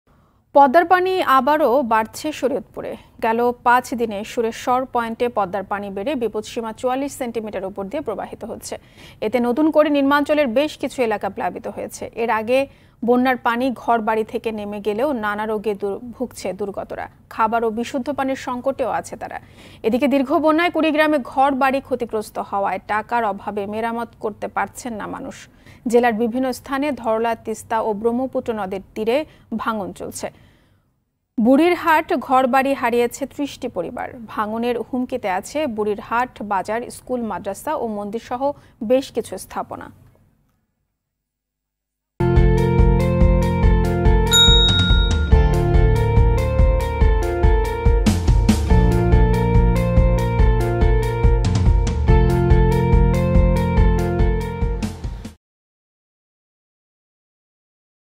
আবারও বাড়ছে পদ্মার পানি। হুমকির মুখে মসজিদ, মন্দির, স্কুল, মাদ্রাসা | Rtv News
গেল ৫ দিন আগেও শরীয়তপুরের সুরেশ্বর পয়েন্টে পদ্মা নদীর পানি কমে বিপদসীমার ১১ সেন্টিমিটার নিচ দিয়ে প্রবাহিত হচ্ছিল। হঠাৎ ৫ দিনের ব্যবধানে তা বেড়ে বিপদসীমার ৪৪ সেন্টিমিটার ওপর দিয়ে পানি প্রবাহিত হচ্ছে। ফলে যে সকল এলাকা থেকে পানি নেমে গিয়েছিল তা আবার নতুন করে প্লাবিত হচ্ছে।

- Team RTV